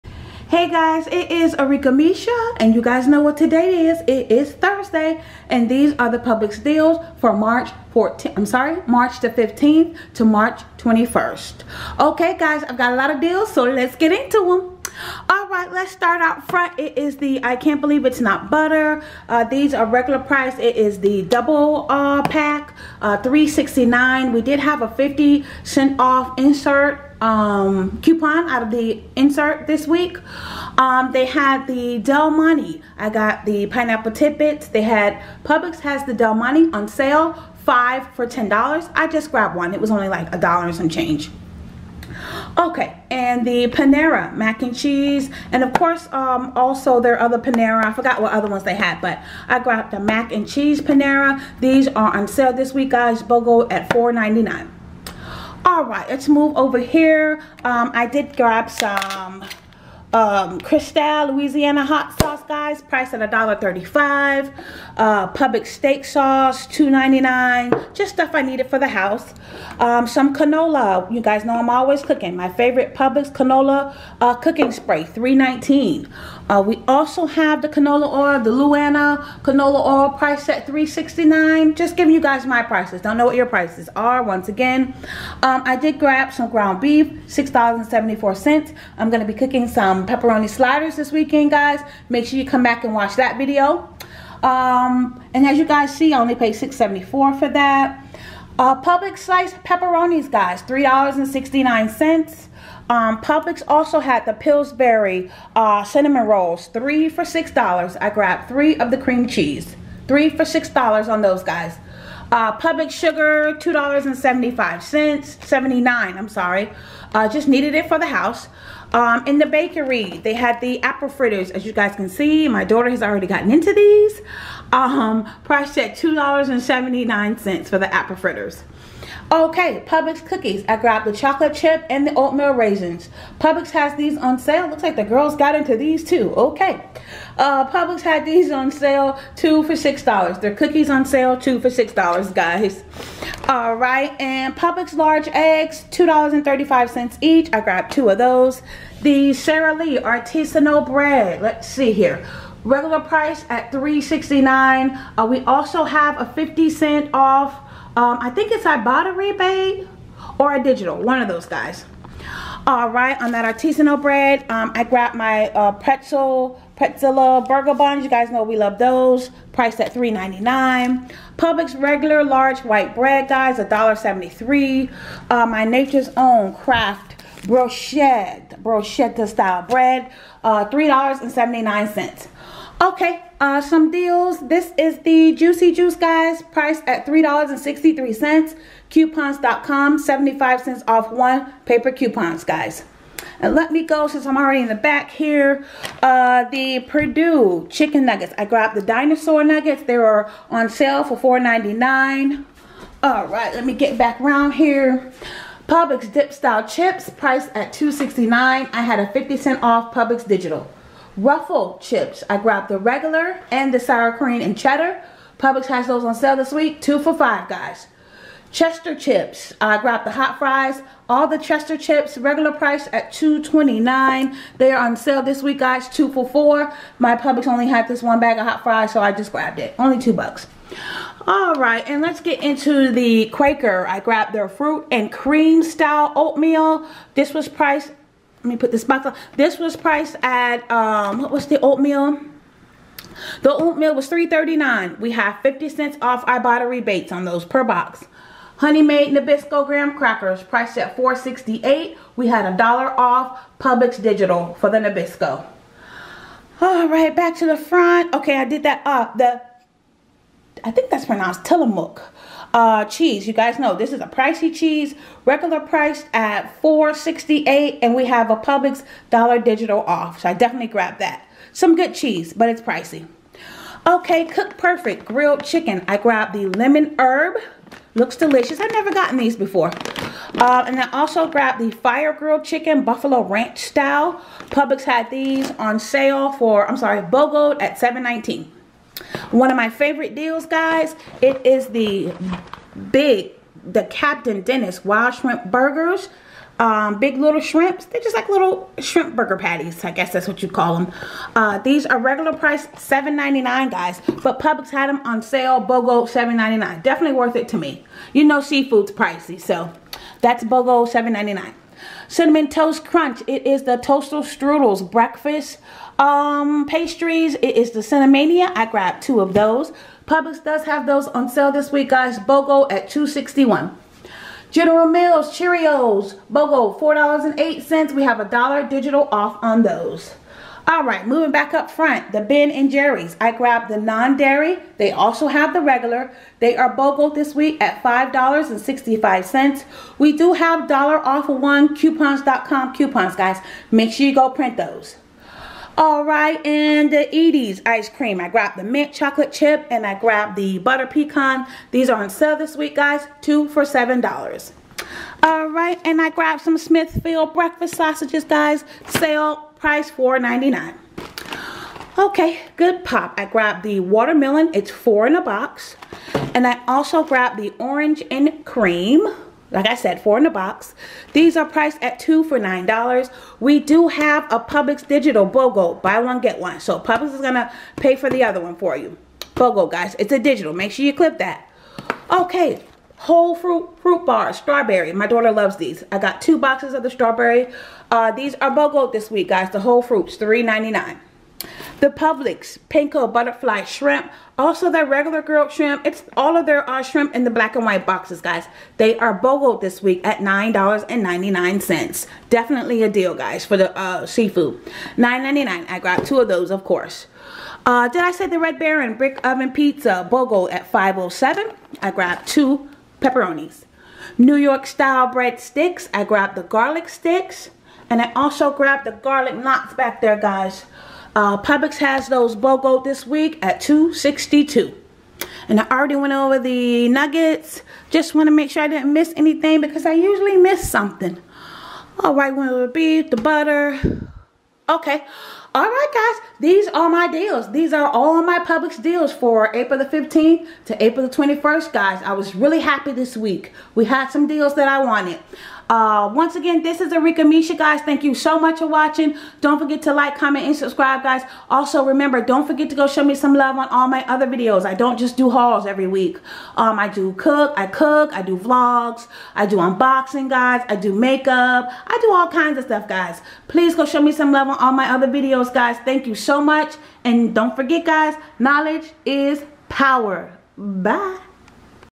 Publix Weekly Grocery Haul 4-15-21 to 4-21-21| Great Bogos | Arickamisha
I was so happy today Publix had so many Bogo deals I couldn't stop myself they had amazing ice cream deals and frozen deals.